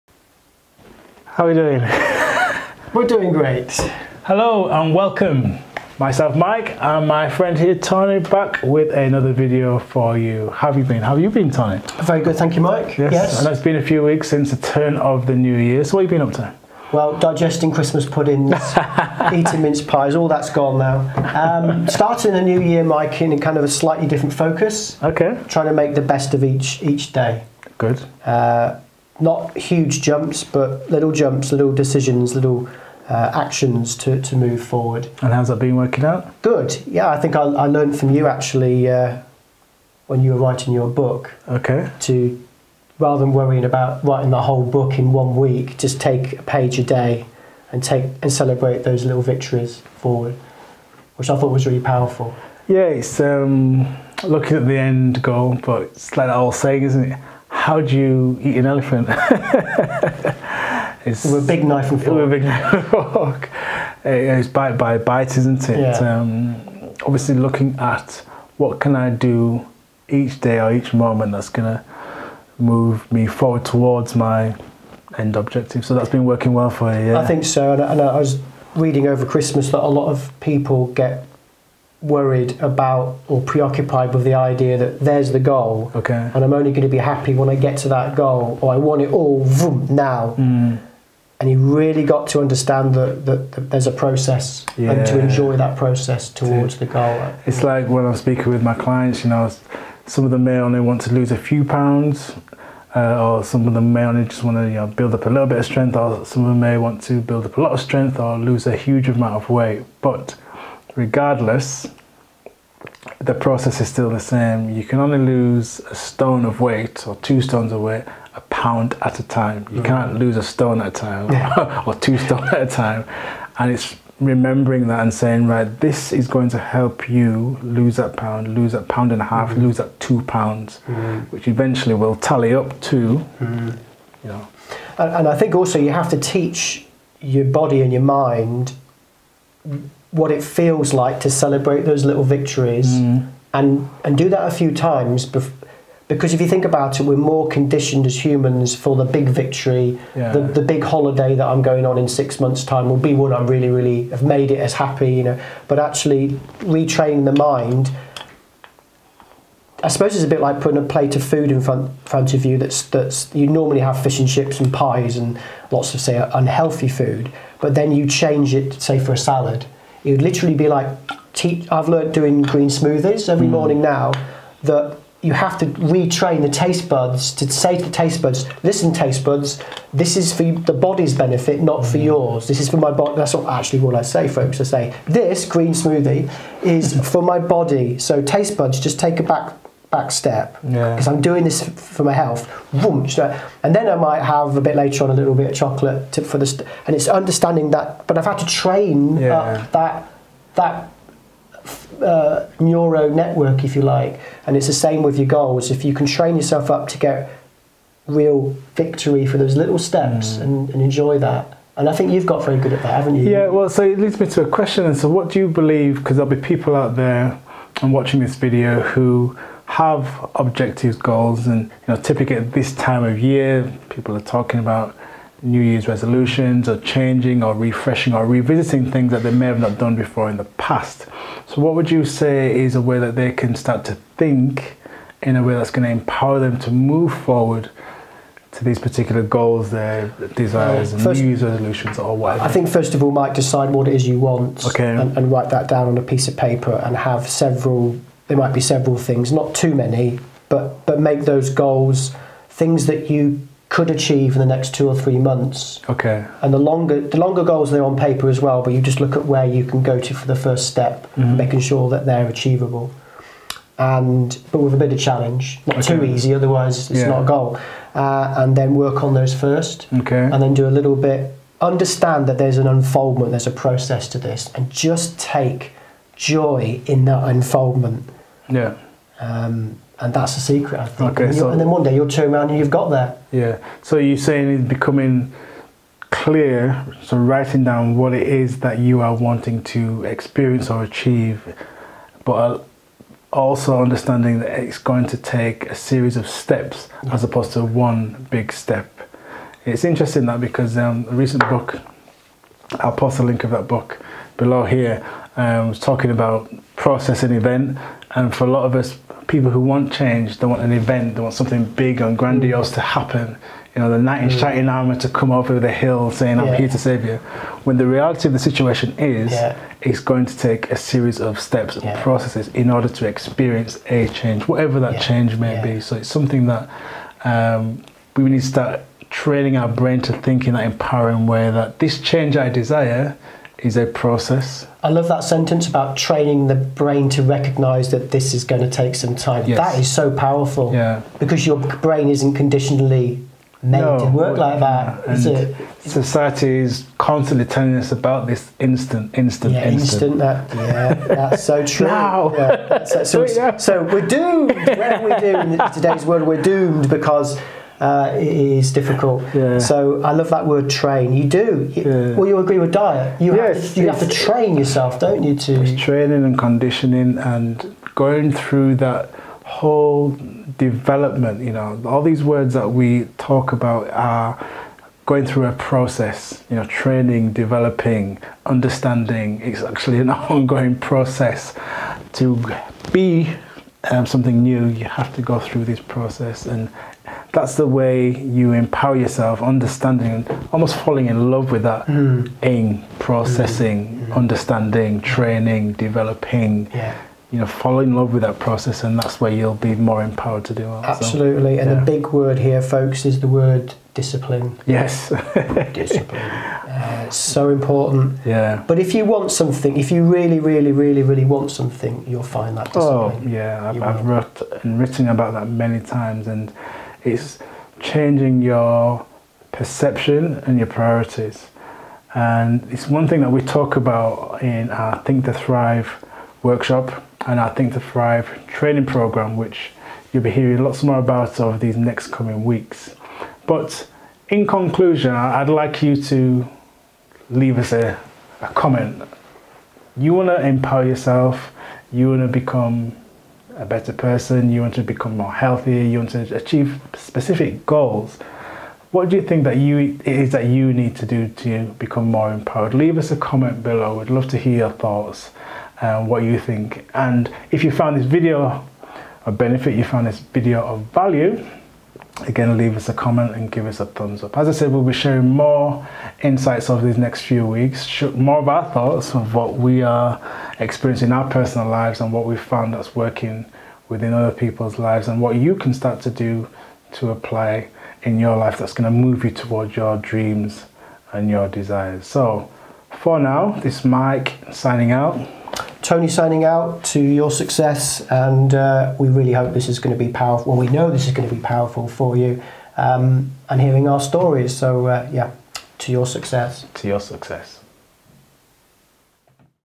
Wellbeing Coaching: How To Become Empowered & Reach Your Goals
Author & Fitness Coach Mike Hendricks & Personal Development coach Tony Mallet Leeds

Inspiring busy frustrated people to live the life they truly desire!

-Online Coaching
-Apparel
-EBooks

Thinking of Improving Your Health?